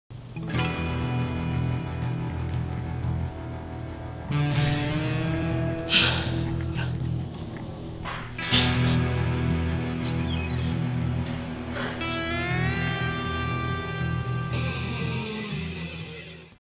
Trailer-AOC
Here is the sneak peak trailer for our skit AOC. Enjoy.